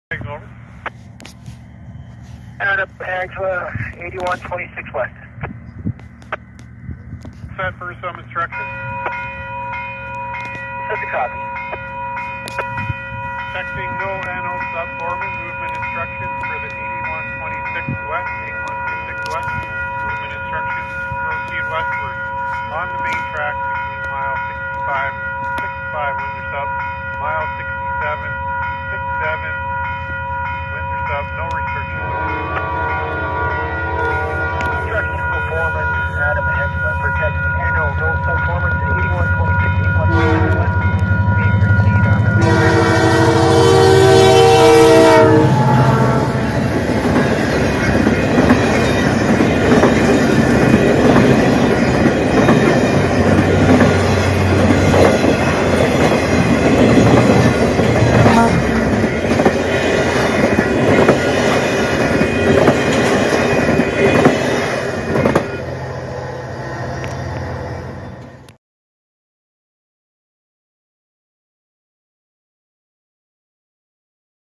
THURSDAY DECEMBER 15TH 2022 the cp train going to Windsor
the cp holiday train leaves